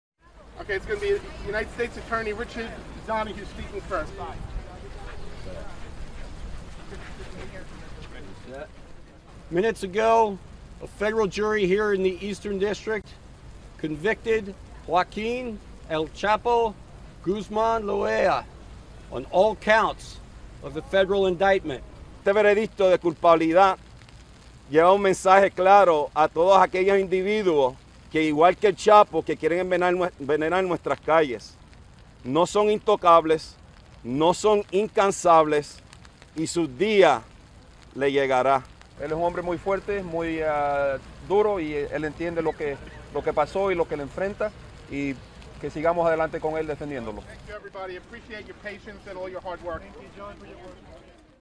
VIDEO Lawyers react after Mexican mobster Joaquin El Chapo Guzman was found guilty
VIDEO: Lawyers react after Mexican mobster Joaquin "El Chapo" Guzman was found guilty by a New York jury of crimes spanning a quarter of a century.

"He was bringing our spirits up," one of his lawyers says at the conclusion of the three-month trial